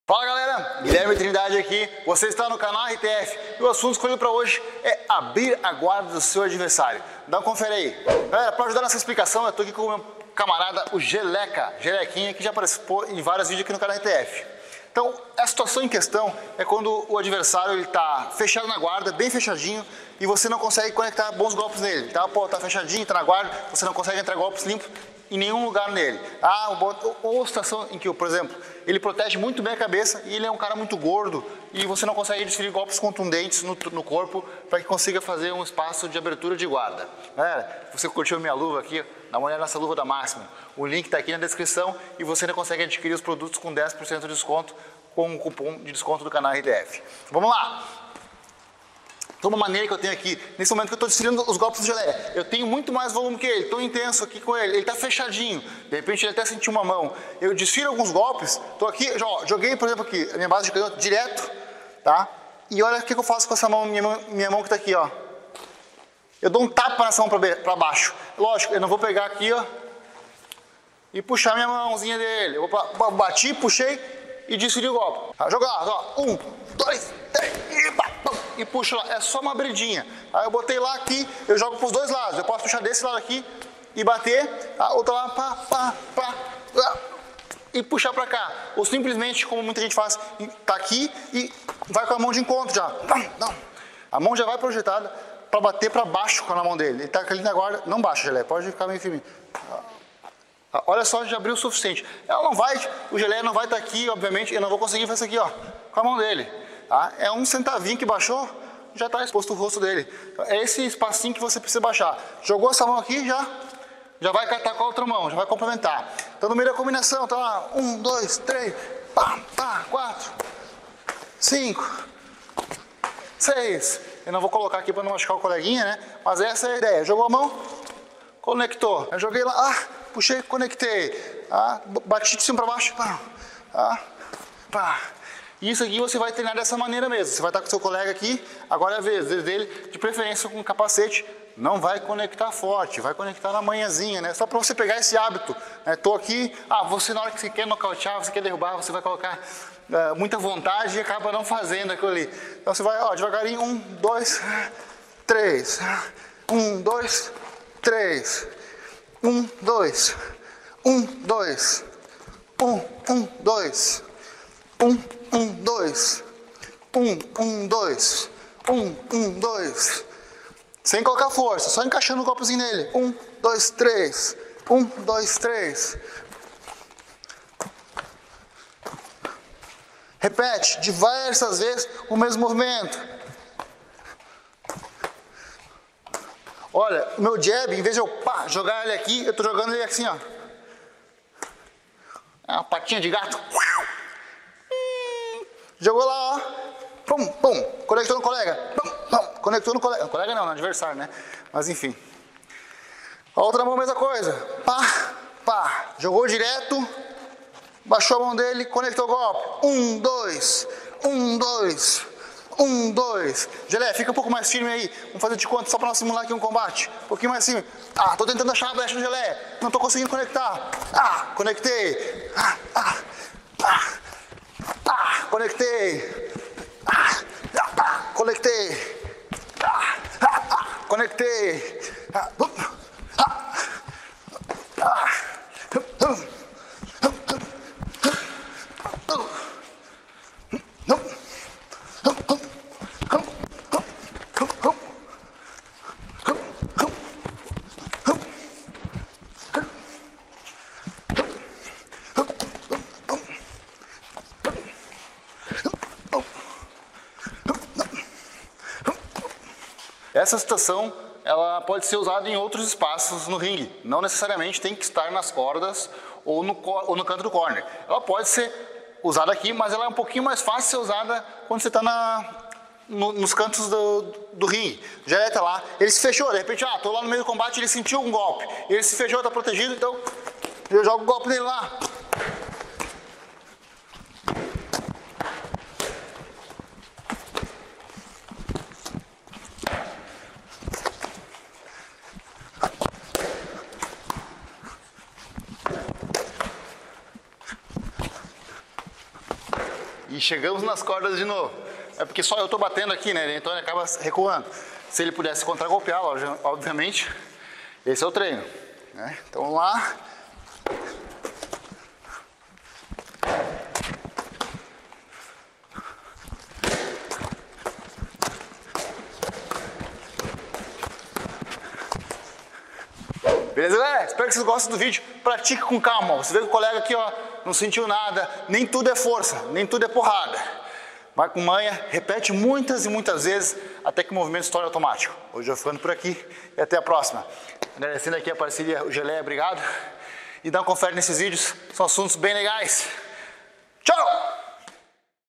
Como abrir a guarda do adversário [ RTF #192 ]
Neste vídeo mostro uma técnica de como conectar golpes na cabeça do adversário quando ele está todo fechado na guarda.